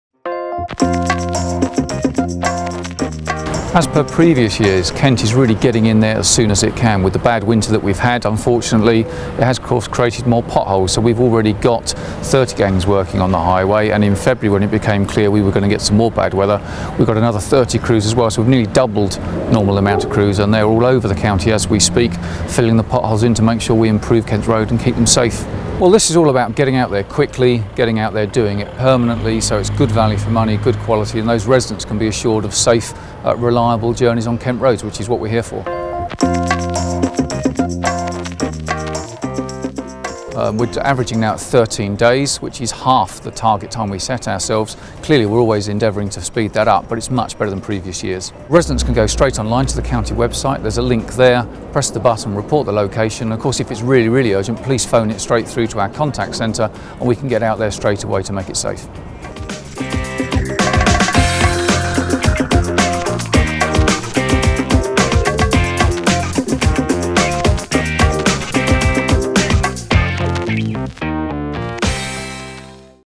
'Find and Fix' Pothole Blitz - Kent County Council
John Burr - KCC director for Highways and Transportation gives an update on 'Find and Fix' 2013